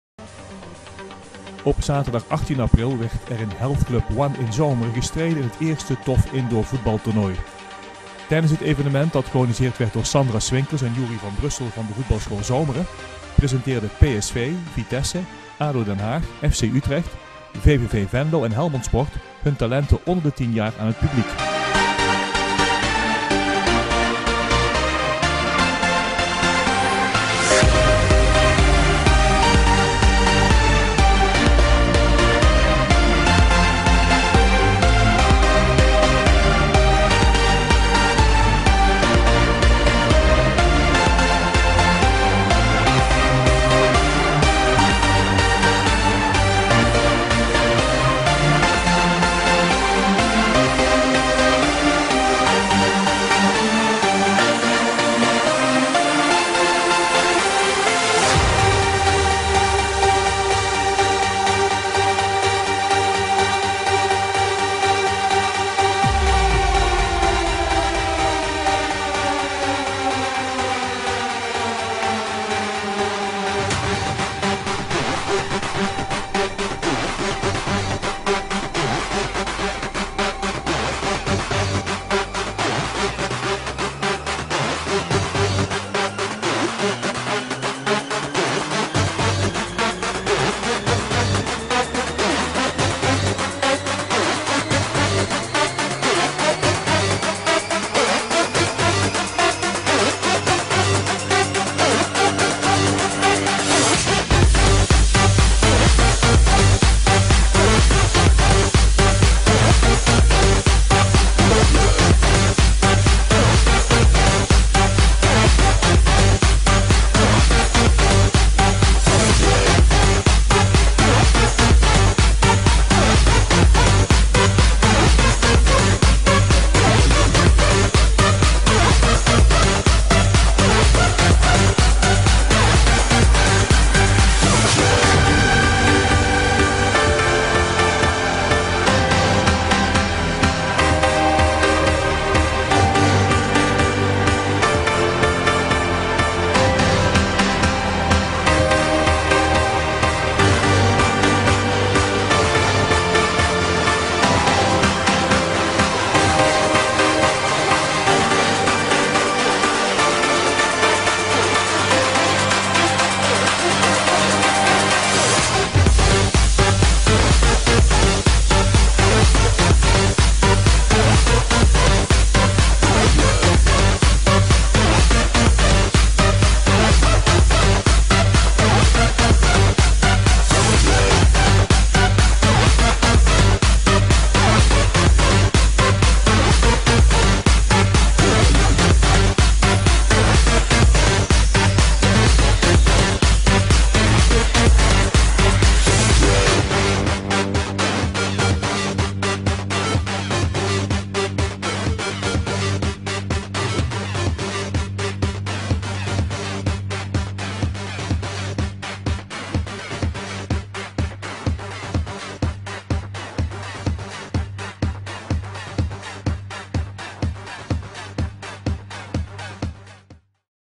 TOFF indoor voetbaltoernooi
Op zaterdag 18 april werd in HealthClub One in Someren het eerste TOFF Indoor Voetbaltoernooi gehouden. Tijdens dit evenement, dat georganiseerd werd door Sandra Swinkels en Youri van Brussel van de Voetbalschool Someren,\ presenteerden PSV, Vitesse, ADO Den Haag, FC Utrecht, VVV Venlo en Helmond Sport hun talenten onder tien jaar aan het publiek.